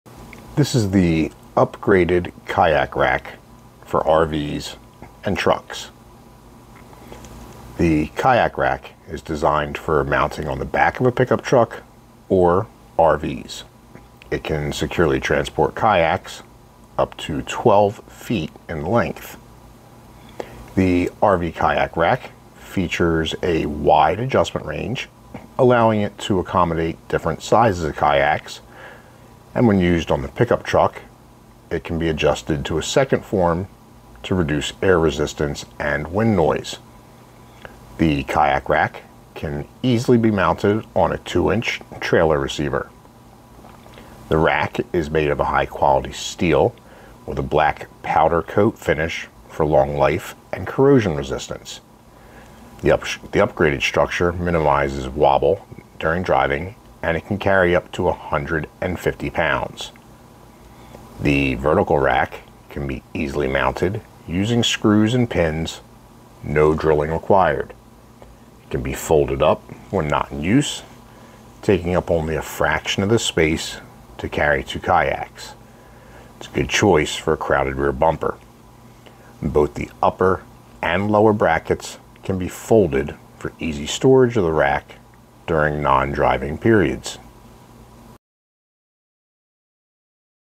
Upgraded Kayak Rack for RV Truck
Kayak rack is designed for mounting on the back of a pickup truck or RV. It can securely transport kayaks up to 12 feet in length.

The RV kayak rack features a wide adjustment range, allowing it to accommodate different sizes of kayaks. When used on a pickup truck, it can be adjusted to a second form to reduce air resistance and wind noise.

 The kayak rack can be easily mounted on a 2" trailer receiver.

The rack is made of high quality steel with a black powder coat finish for long life and corrosion resistance. The upgraded structure minimizes wobble during driving.Can carry up to 150 lbs.

The vertical kayak rack can be easily mounted using screws and pins, with no drilling required. Can be folded up when not in use.
Takes up only a fraction of the space to carry two kayaks. A good choice for a crowded rear bumper.

Both the upper and lower brackets can be folded for easy storage of this rack during non-touring periods.